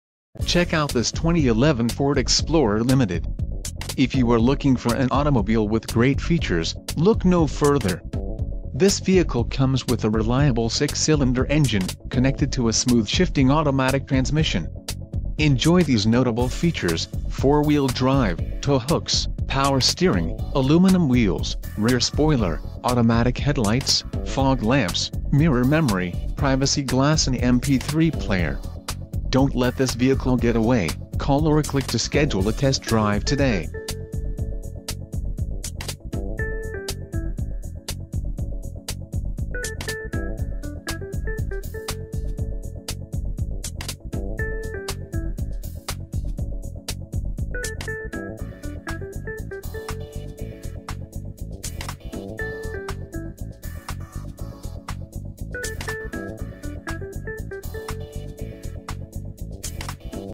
2011 Ford Explorer Limited - GT Auto Sales 2 - Tacoma, WA...
For Sale in Tacoma, WA 984009
 GT Auto Sales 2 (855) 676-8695 ext 101

Sophisticated, smart, and stylish, this 2011 Ford Explorer will jump you into the new era of vehicle technology. With a Gas V6 3.5L engine powering this Automatic transmission, you will marvel at this unique synergy between the forces of mother nature and the laws of physics. It's outfitted with the following options: power folding 3rd row seating, Navigation, premium audio system, panoramic roof, dual rear DVD Entertainment screens, Silver roof rack side rails, 3.39 axle ratio, Olive Ash Dark Brown Woodgrain appliques, 4-wheel drive, Auto on/off headlamps, Body-color pwr folding heated pwr mirrors -inc: integrated blind spot mirrors, LED turn signals, approach lights, driver's memory, Roof mounted antenna, 60/40 split-folding 2nd row seat w/armrest & dual cupholders, Bi-functional projector beam headlamps, Seat belts -inc: pretensioners, energy management system, adjustable height in 1st row.